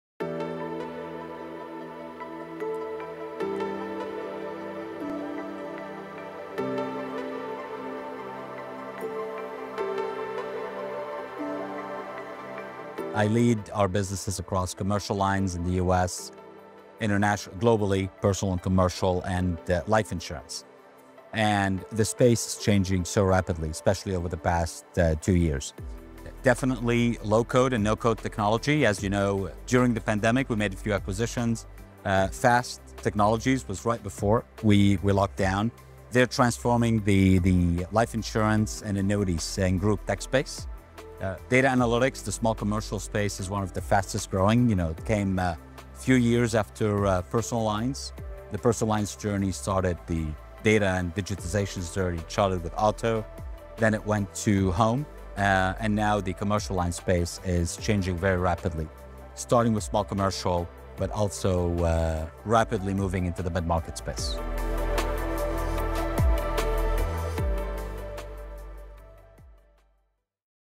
Maroun Mourad, President of Global Underwriting, shares his insights on the top insurance trends
InsureTech Connect Interview with Maroun Mourad, President Global Underwriting at Verisk -  Part 1 of 4